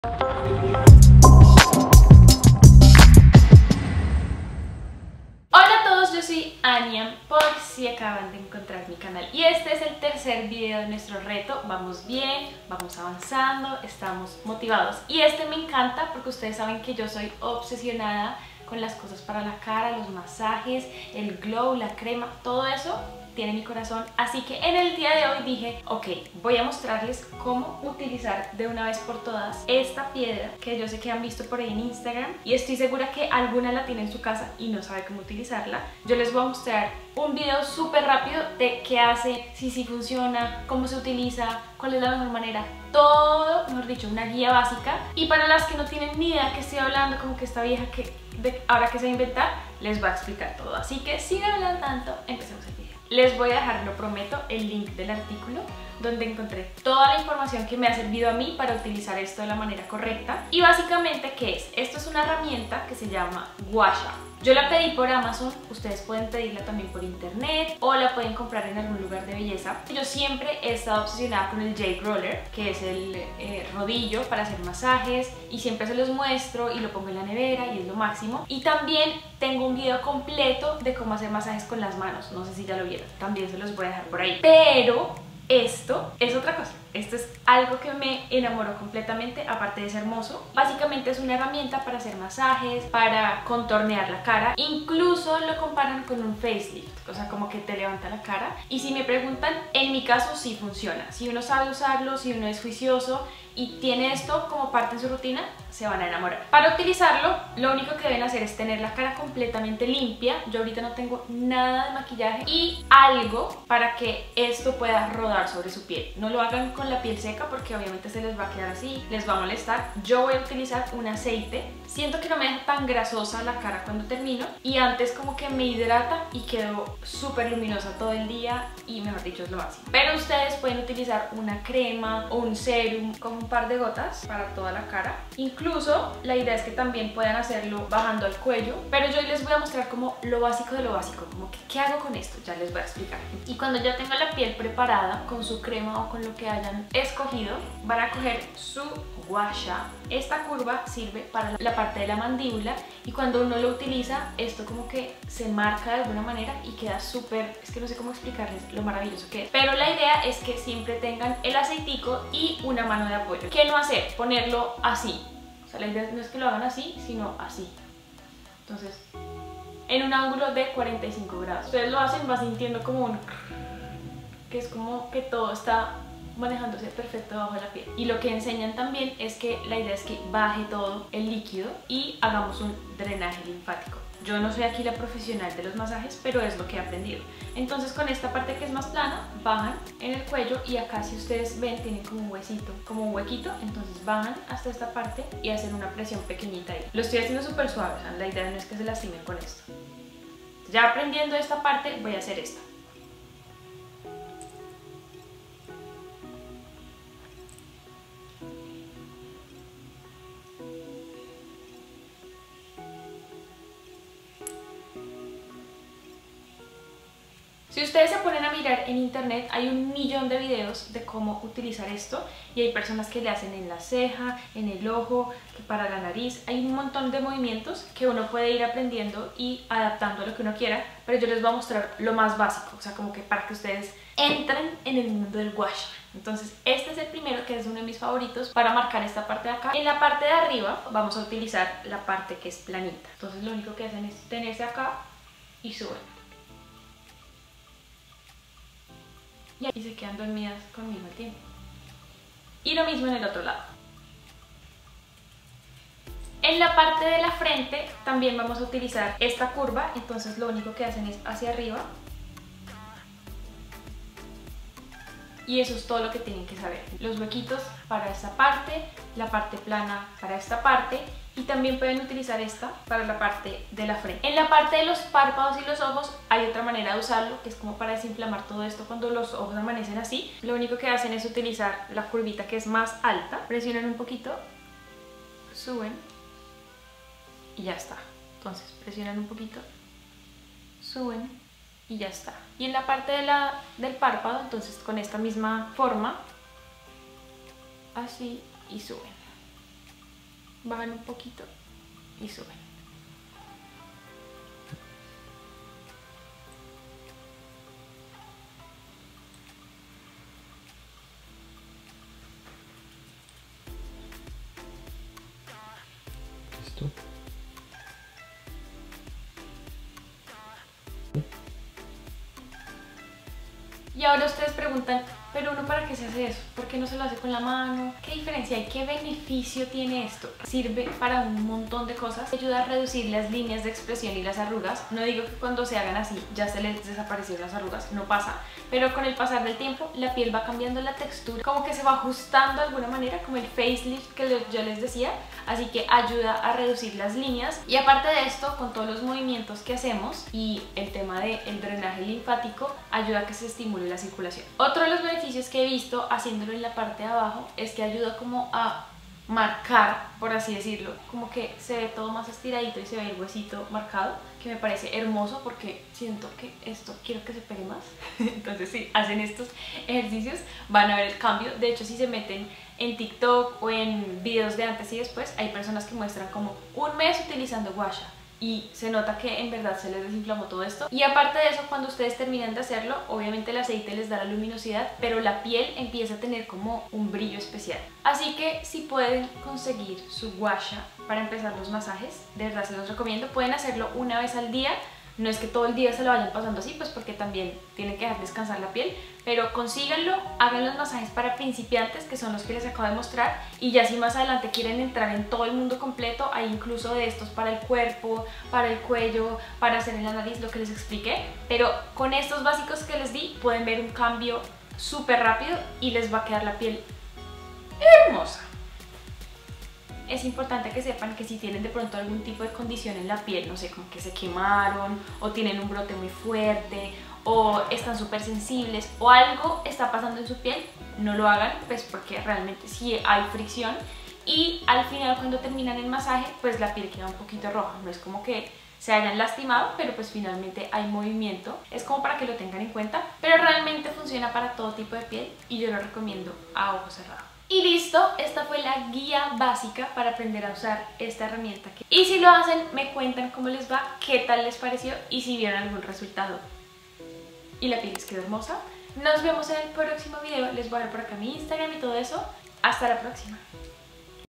Herramienta para moldear el rostro que todas deberían probar! ✨Aniam
Guía básica para usar la Gua Sha, paso a paso y beneficios!